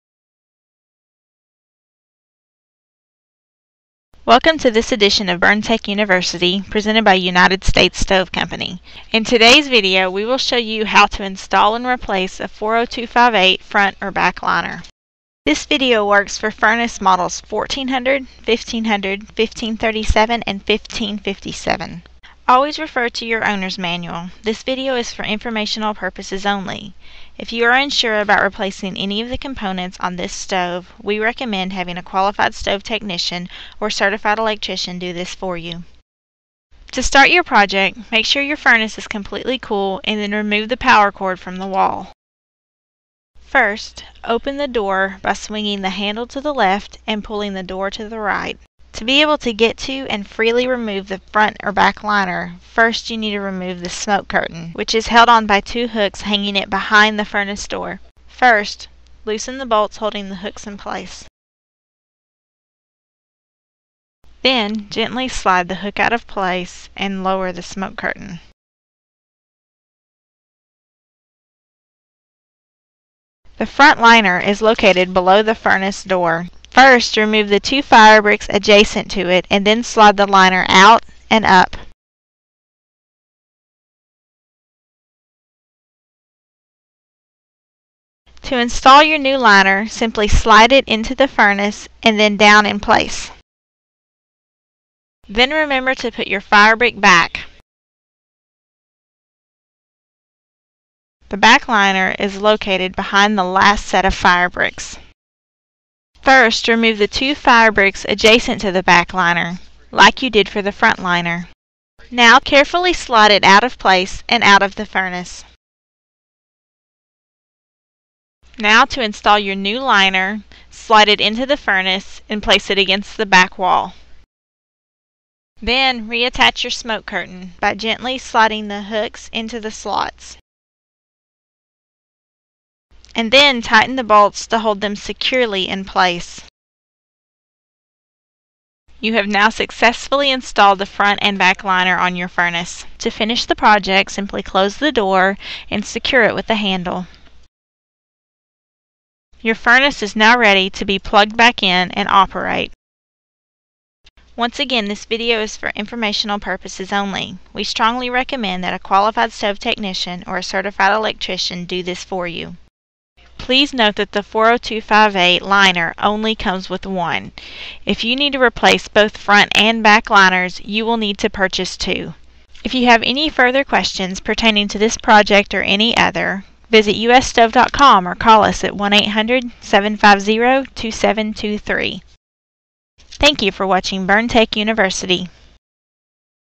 Front And Back Liner (40258)
Video showing how to replace the 40258 front and back liner.